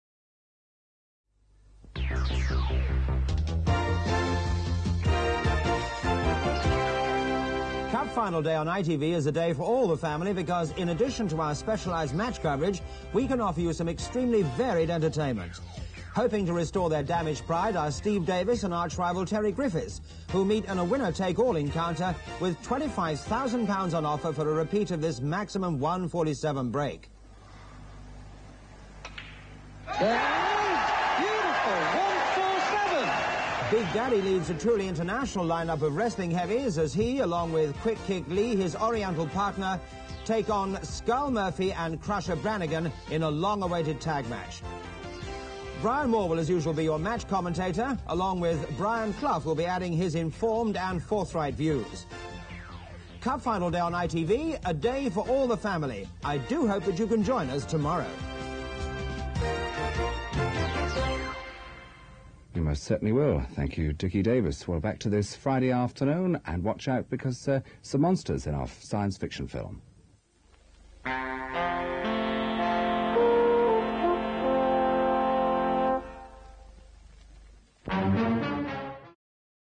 21 May 1982 Thames - FA Cup final day trailer
Dickie Davies previews ITV's day of Cup Final action. Spurs and QPR played out the final that year. Thames then go into an old sci-fi movie.